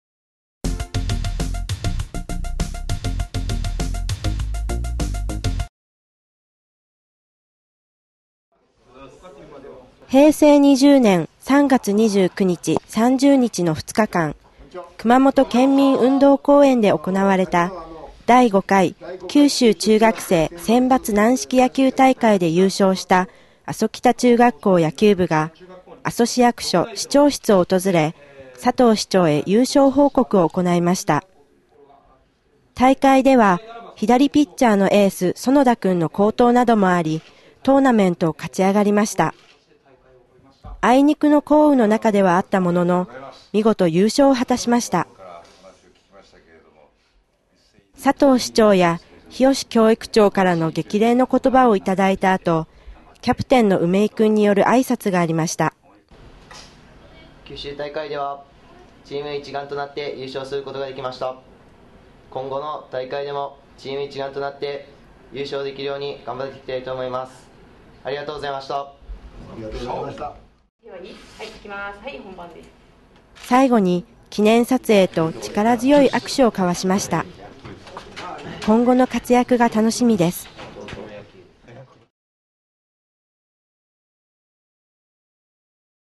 平成20年阿蘇北中野球部が九州大会優勝報告
阿蘇北中学校野球部が九州大会優勝の報告に市長室を訪れました